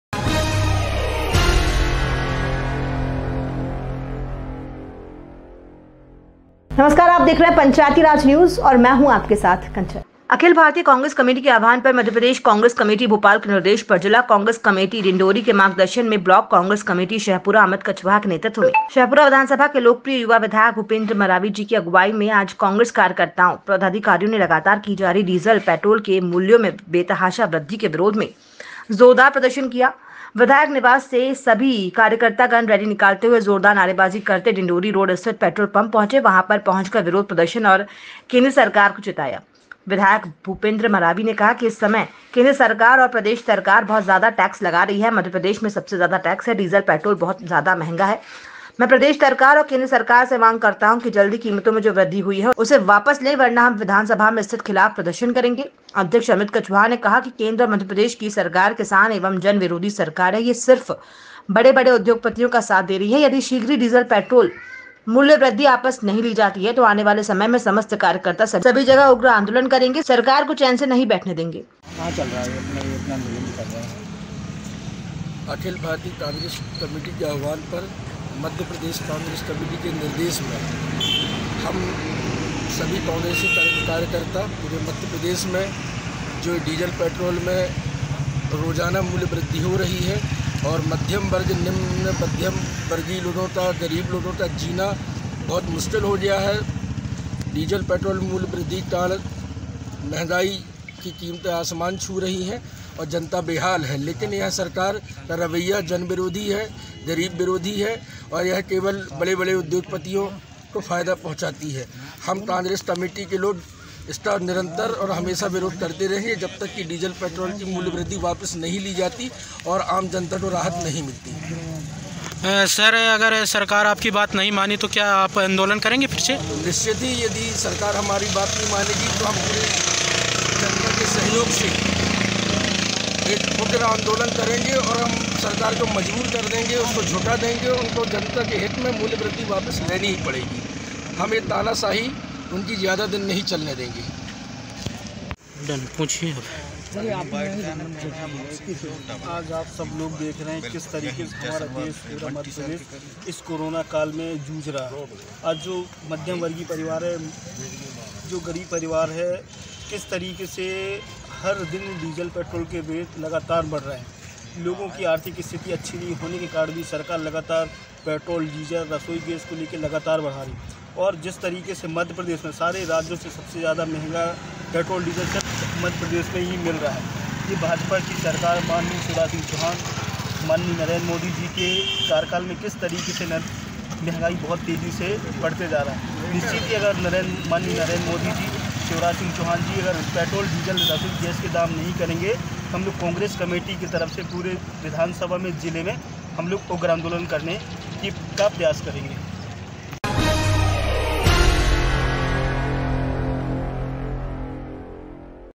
पंचायतीराज न्यूज़ 12 जून डिंडौरी में मूल्य वृद्धि के विरोध में कांग्रेस का हल्लाबोल
जुड़े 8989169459 समाचार ,विज्ञापन हेतु